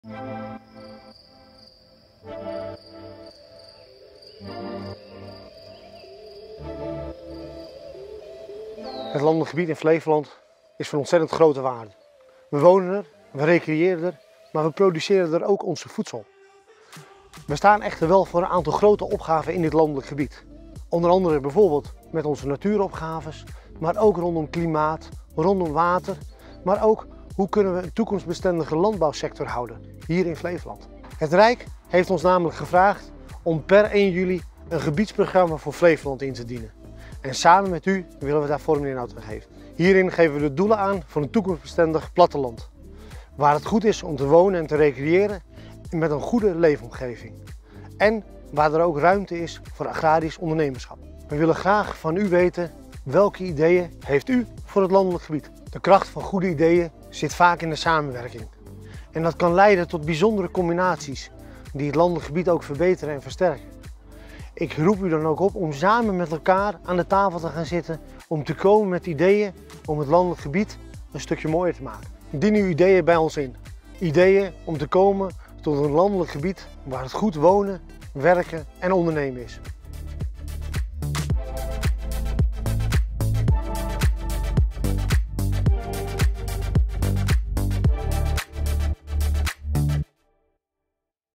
Provincie Flevoland - Ons platteland van morgen zoekt ideeën
Gedeputeerde Harold Hofstra nodigt uit om ideeën in te dienen voor een toekomstbestendig landelijk gebied Flevoland.